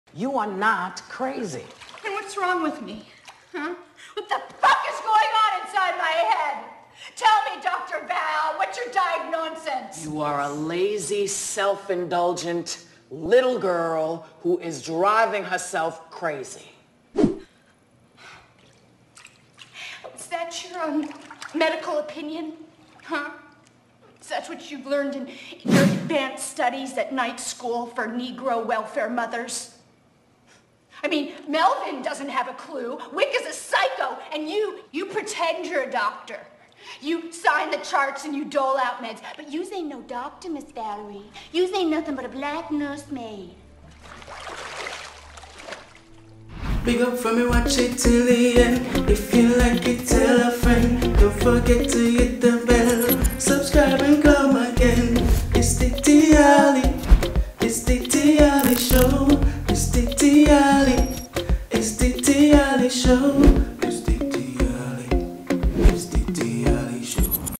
GIRL INTERRUPTED "what's your diagnonsense?" - DIHALLI RE-CREATION
1. Re-creating the best scenes from girl interrupted whoopi goldberg vs Winona Ryder
2. Girl inturrepted "You are not crazy"
3. Girl inturrepted "what's your diagnonsense?"
4. Girl, Interrupted is a 1999 American psychological drama film directed by James Mangold, and starring Winona Ryder, Angelina Jolie, Clea DuVall, Brittany Murphy, Whoopi Goldberg amongst others
5. What's wrong with Daisy in Girl Interrupted?
6. Girl interrupted whoopi goldberg vs Winona Ryder arguement

Thank you for sharing you're precious time with me, I do hope I earned it and you enjoyed my video.
Teamwork makes the dream work. Together we can make it happen.